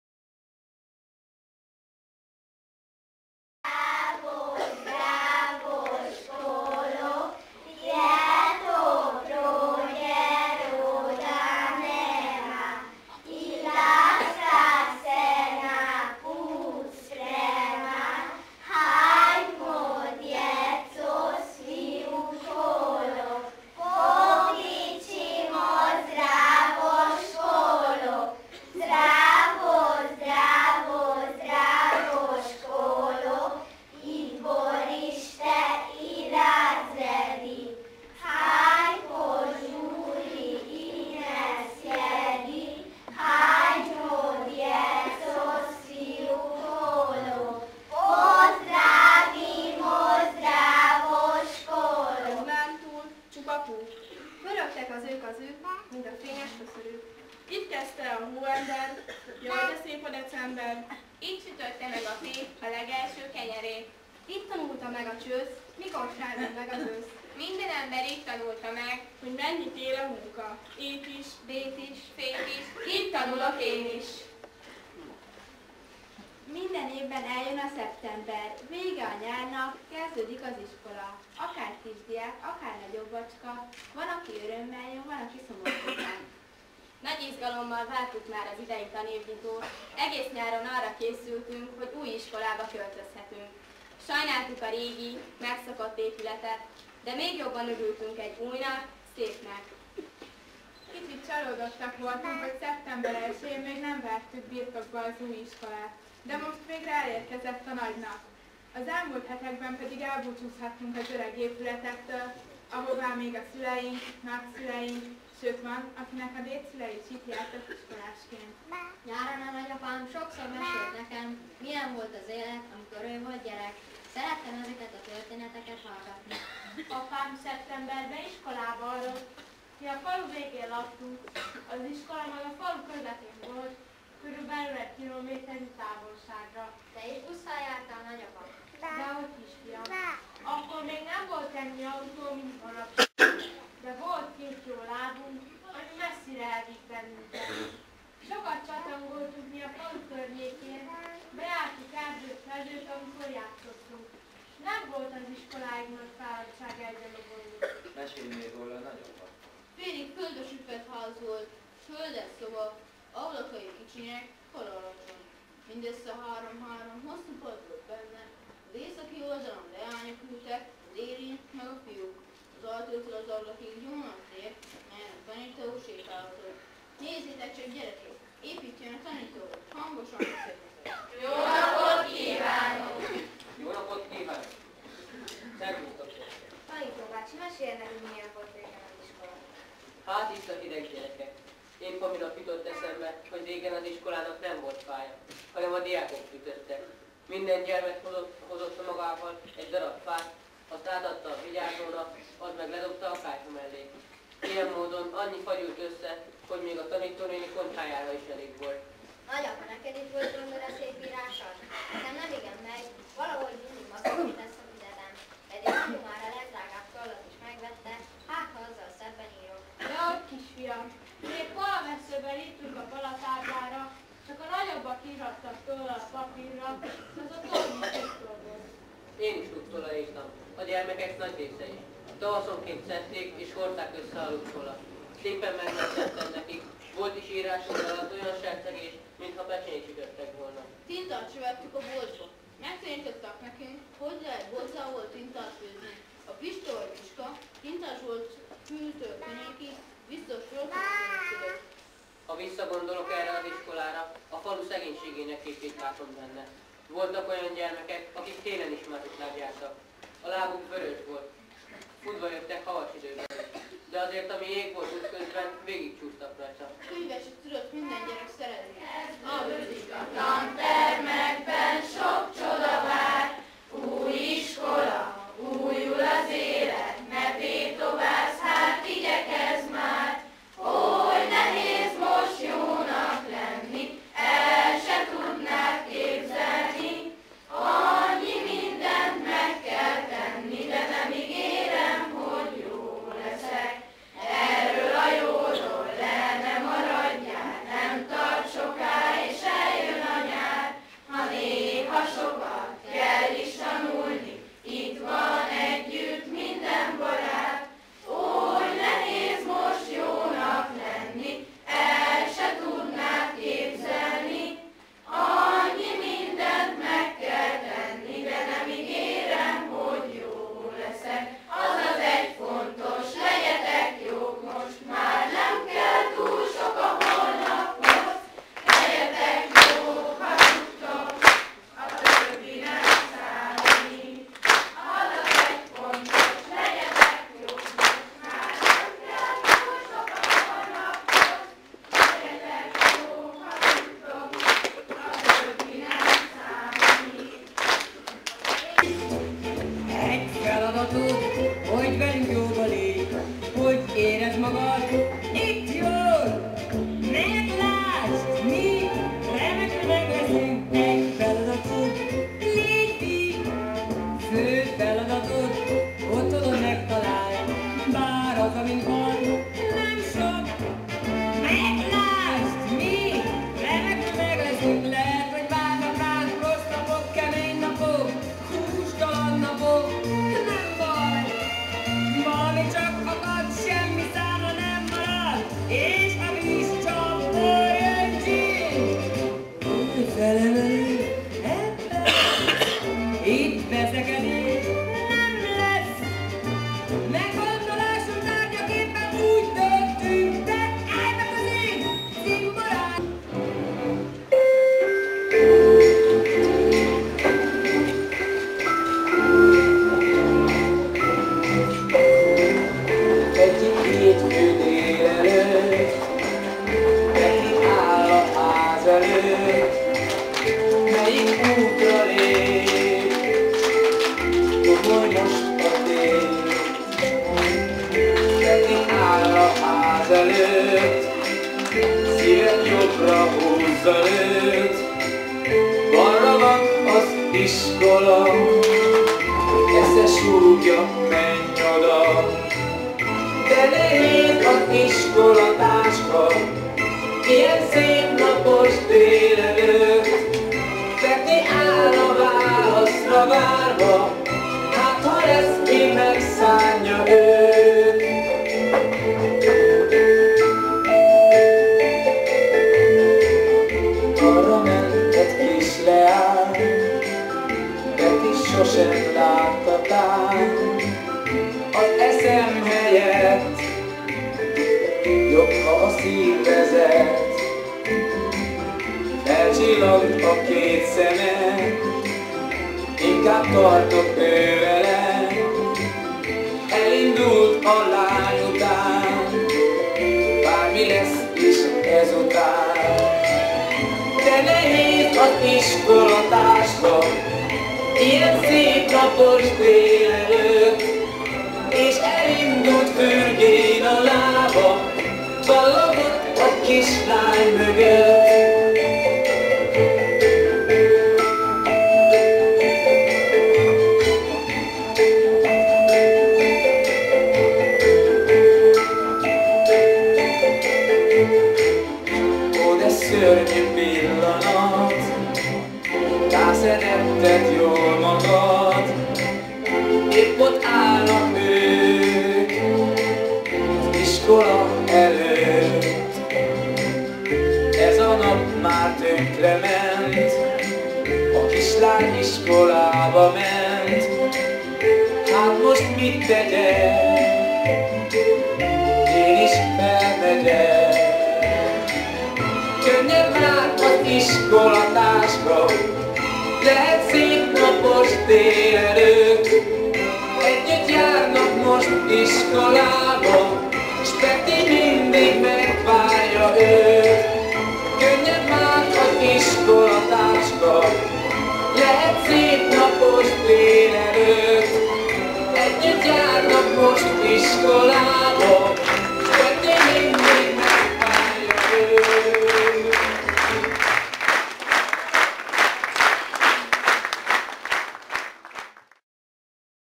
Becsehely: részletek a diákok műsorából az avatón
Egy gyönyörű iskola avatóján előadott műsorból láthatóak a részletek. (2010. november 21.)